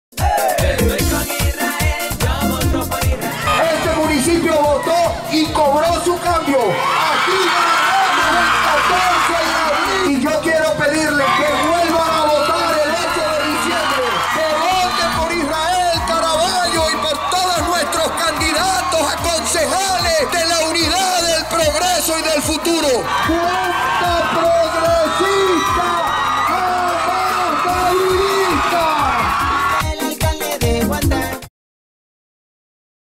El 8D Guanta Vota Unidad
Lo Dijo Henrique Capriles en Guanta: "Yo les pido a todos los que votaron por mí, que el 8D lo hagan por Isrrael Caraballo"